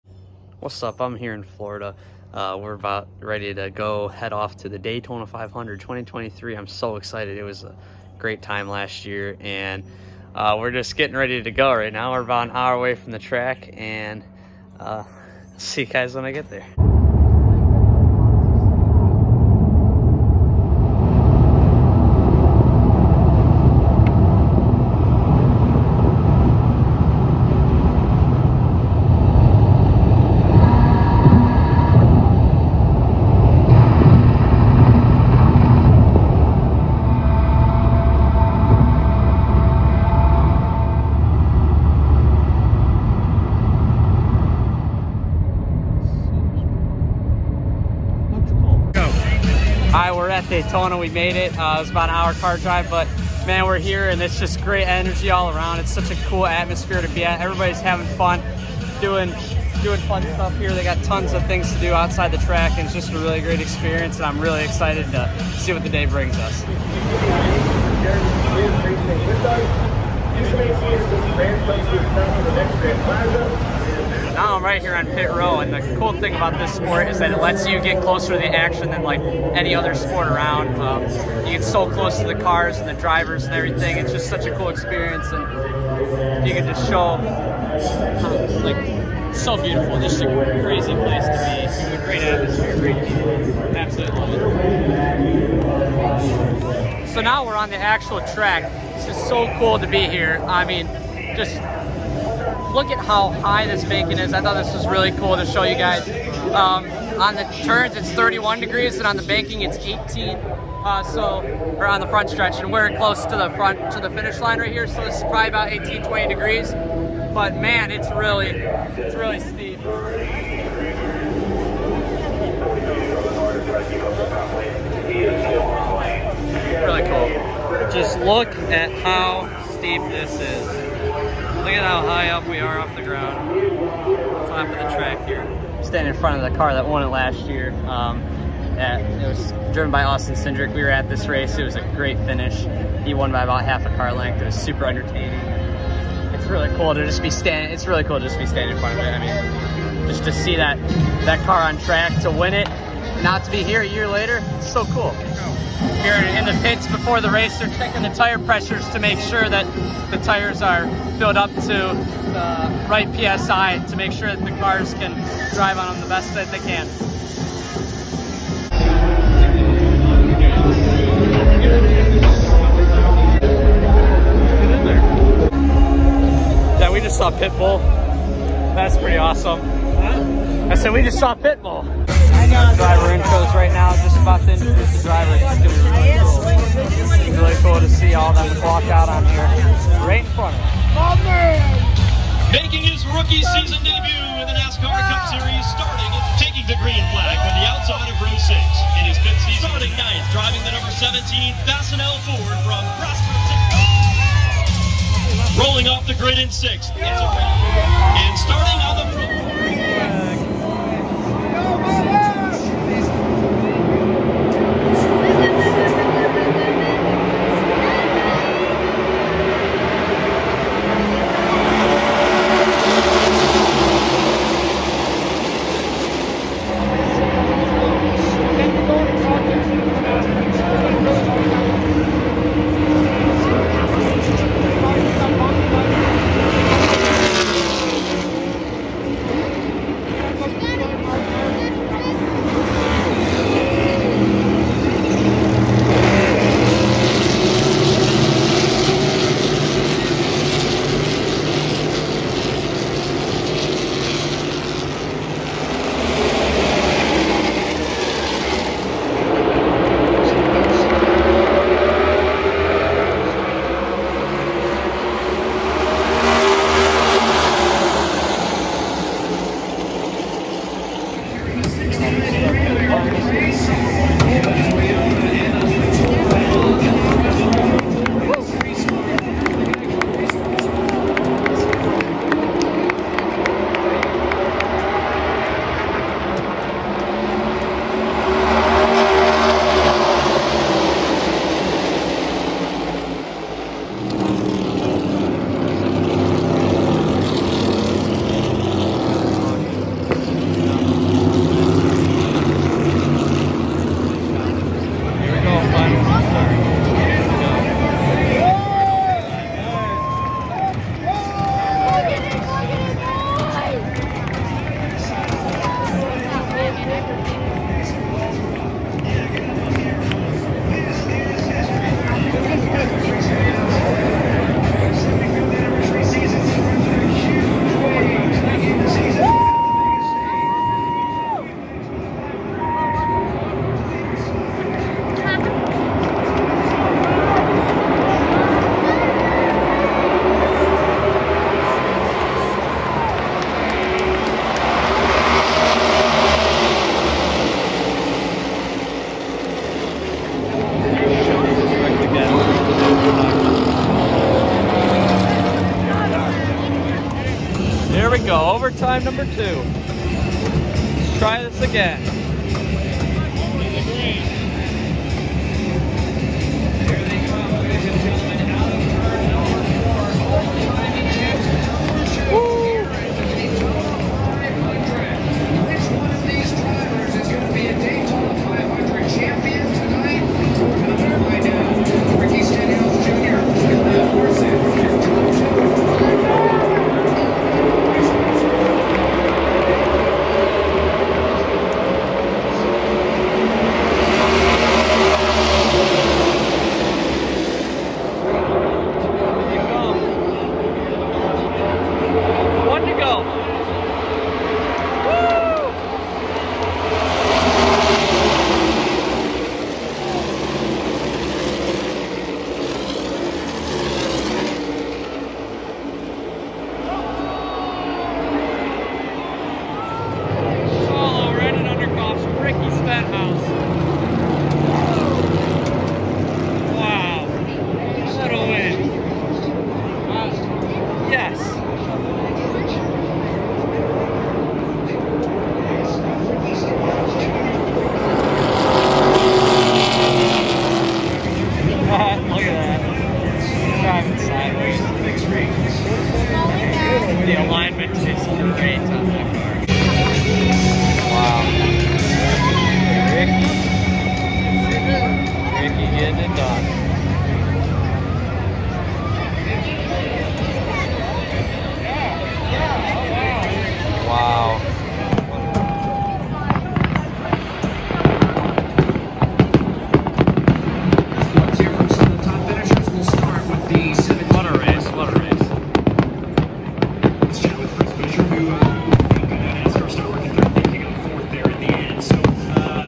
Daytona 500 Vlog
Editor Noah Stockoski records his experience at the 2023 Daytona 500!!!